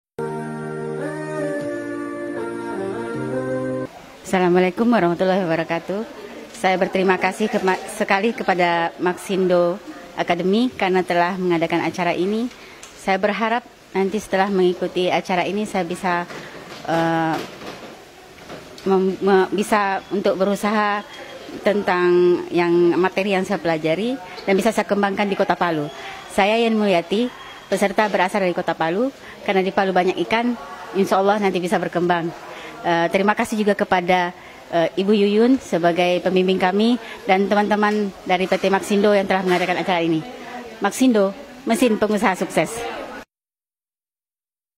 Mulyati, Testimoni Maksindo Academy, Training Usaha Frozen Food
Apa Kata Peserta Training Usaha Frozen Food

Training usaha frozen food telah banyak diikuti oleh para calon pengusaha yang ada di Indonesia. Salah satunya adalah Ibu Mulyati asal Palu. Alasan Beliau mengikuti training usaha frozen food di Maksindo adalah untuk mengetahui tehnik-tehnik dalam membuat frozen food. Harapan beliau setelah mengikuti training usaha ini adalah untuk mengembangkan materi yang dia dapat dan membuka usaha karena di Palu banyak ikan.

Anda tertarik mengikuti training usaha seperti ini, untuk informasi lebih lengkap mengenai training - training usaha dari Maksindo. Maka silahkan kunjungi